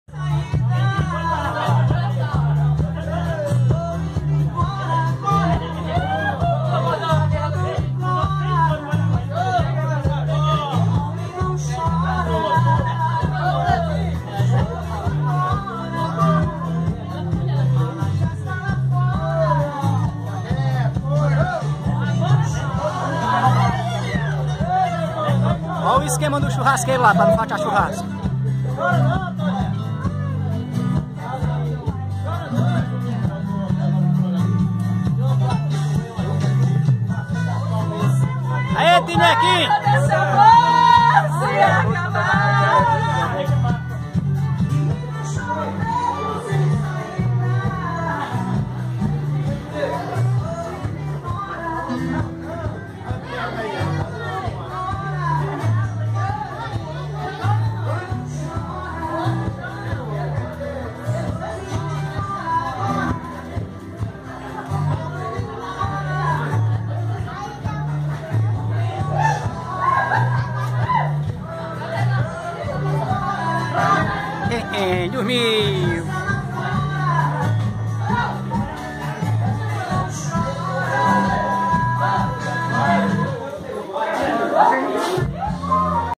Tiago Medeiros,, família Sousa
Casamento de Tio Cícero ano  2016